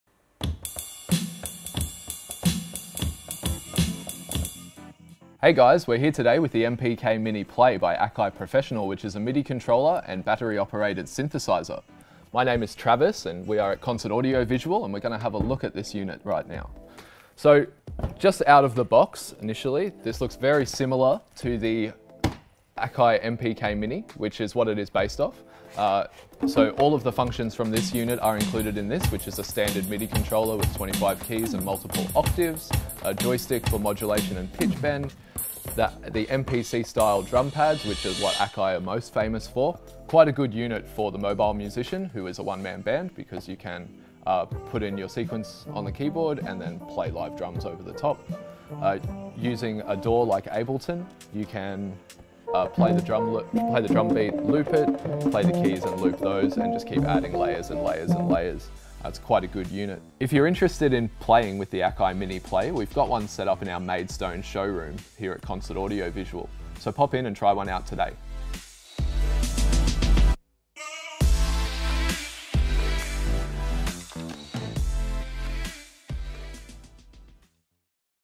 Concert Audio Visual - Akai MPK MINI PLAY Review
Need a portable rig?
We go in-depth with this product to show you why it's an essential!

The MPK Mini Play from Akai Professional expands on the already versatile controller and adds:

📍128 synth sounds

📍10 drum kits built-in speaker

📍with battery-powered options.

Equally impressive at home, in the studio or on stage!

Come in to the Concert Audio Visual Showroom in Maidstone to try one out for yourself!

2/61 Wattle Road,
Maidstone, 3012